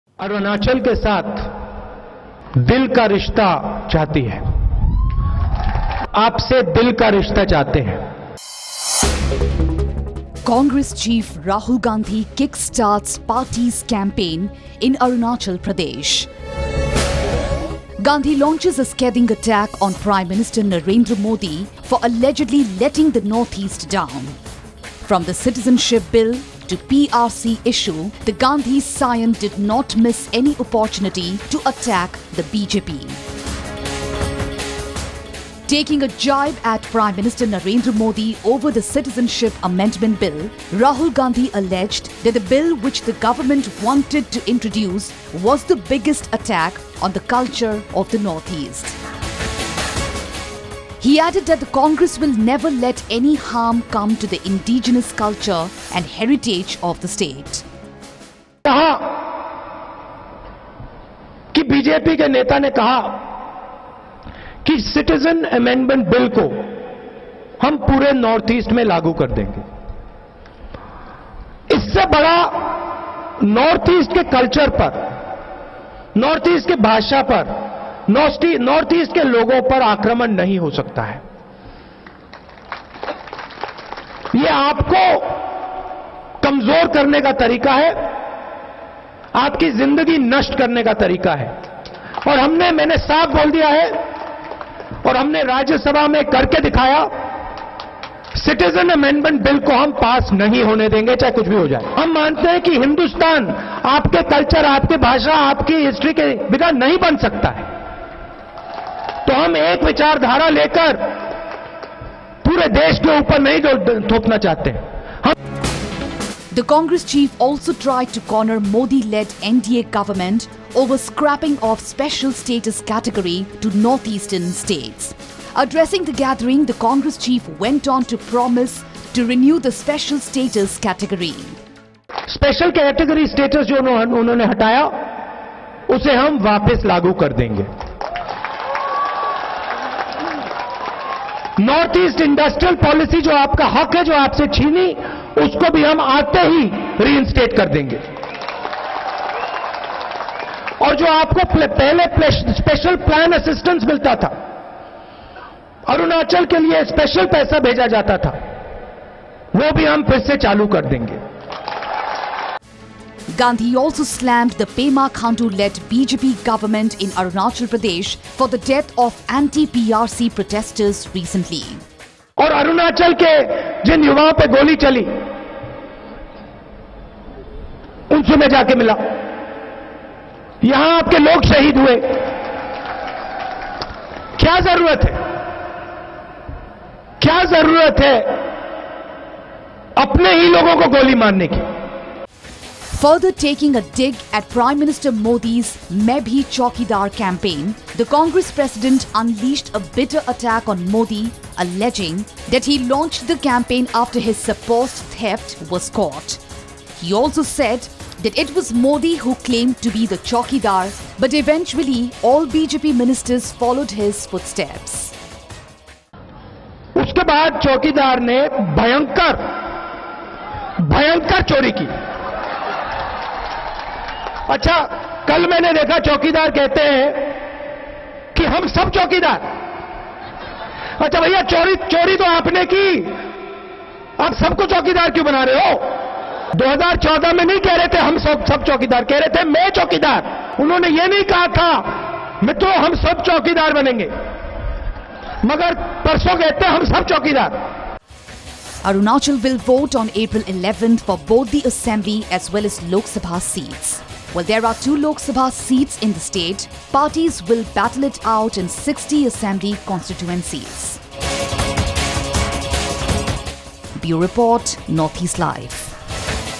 Rahul Gandhi says Congress has 'Dil ka rishta' with Arunachal; promises to restore special status
Congress chief Rahul Gandhi kick started the party’s campaign in Arunachal Pradesh today even as he launched a scathing attack on Prime Minister Narendra Modi for allegedly letting the Northeast down.
Gandhi slammed the ruling BJP government on several grounds - right from the Citizenship Amendment Bill to the PRC issue.